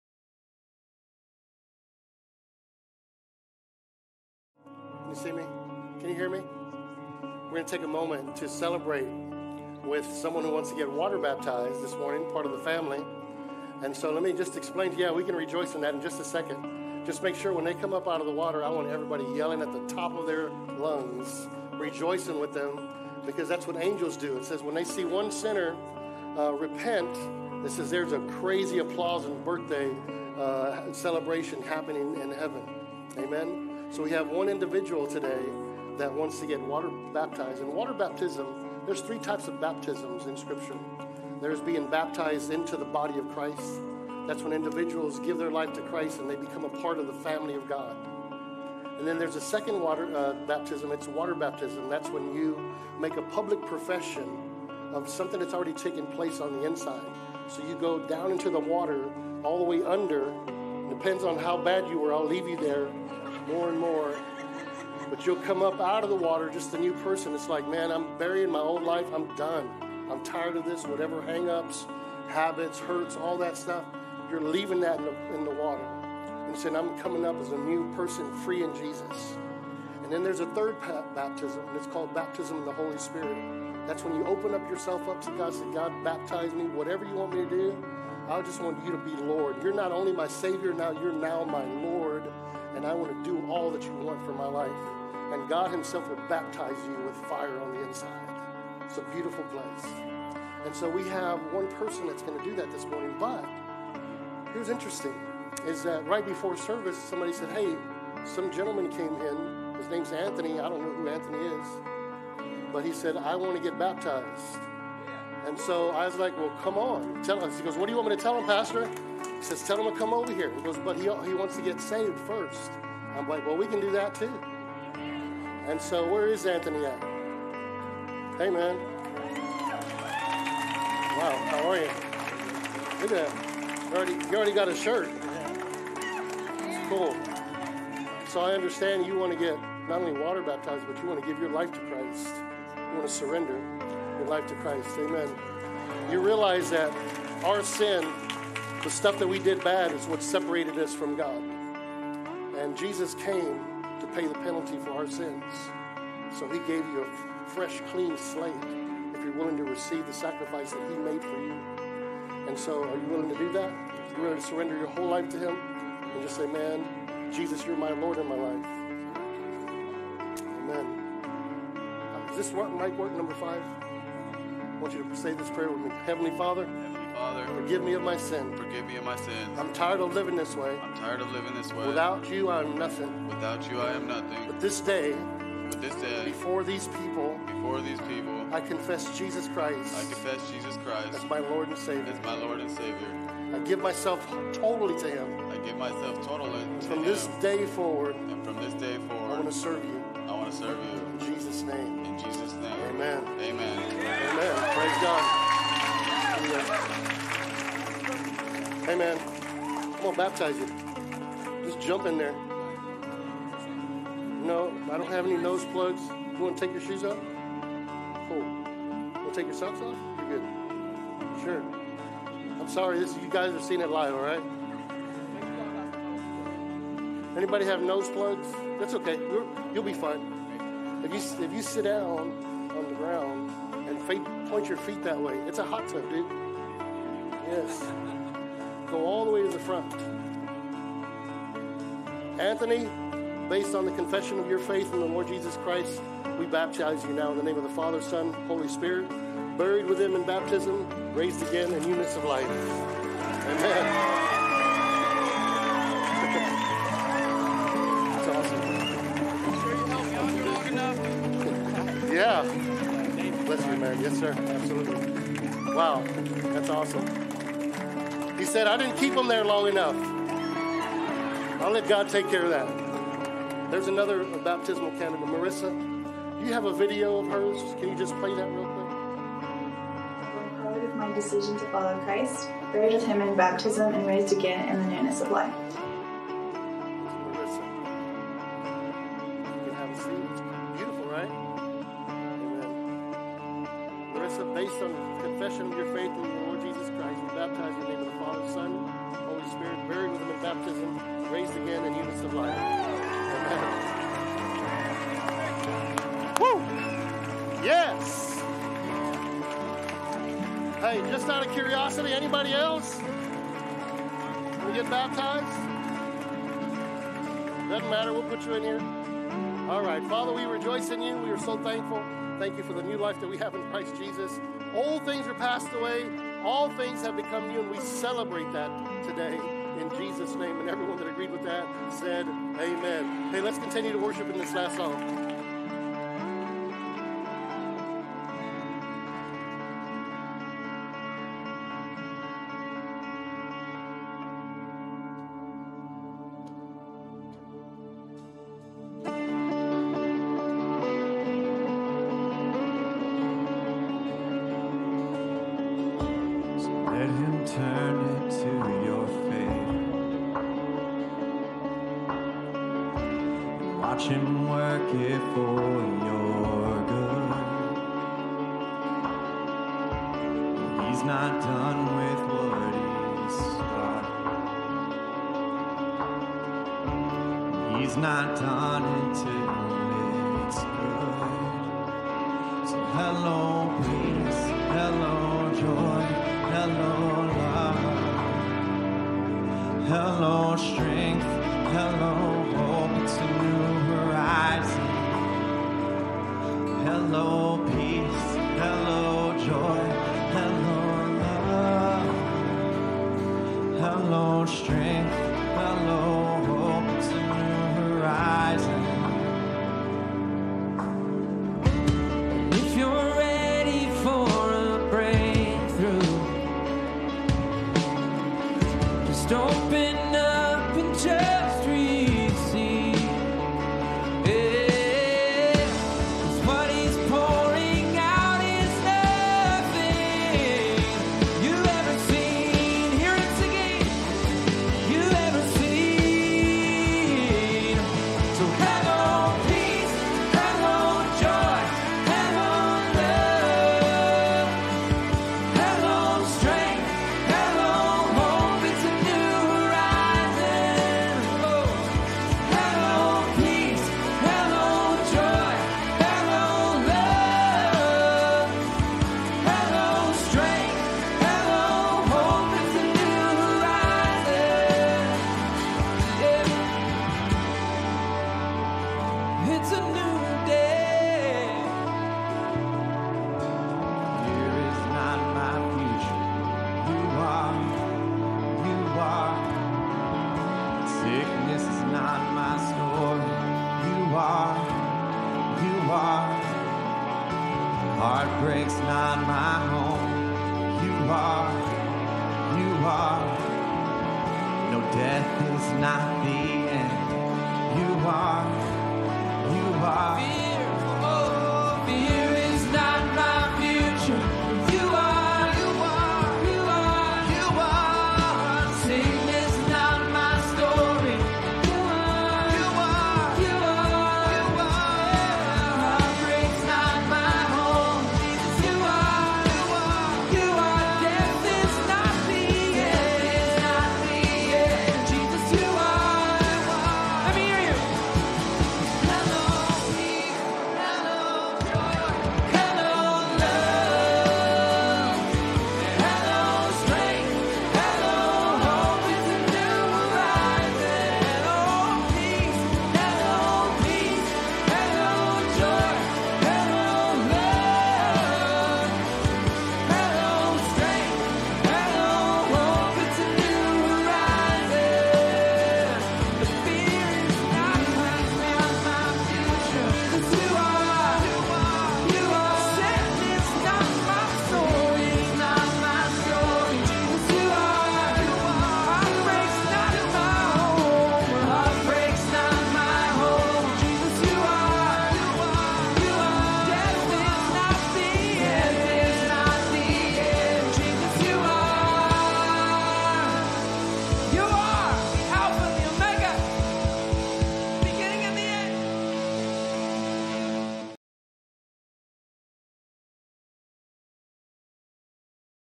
1.29.23 | Water Baptism
Once you have made the decision to follow Christ, water baptism is the next step in your spiritual journey. We congratulate those who were able to rejoice in this next step this morning!